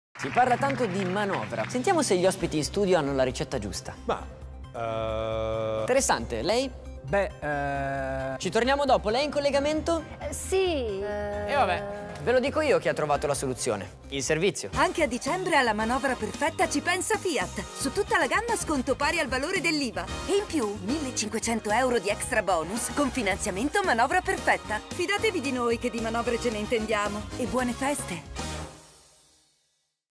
Fiat - La manovra perfetta di Natale
La manovra perfetta esiste, e non è quella di Babbo Natale. Fino al 31 dicembre su tutta la Gamma Fiat, sconto pari al valore dell’IVA. E in più, con finanziamento Manovra Perfetta, 1.500€ di extrabonus! Oltre oneri finanziari. TAN 6,45% e TAEG 9,57%.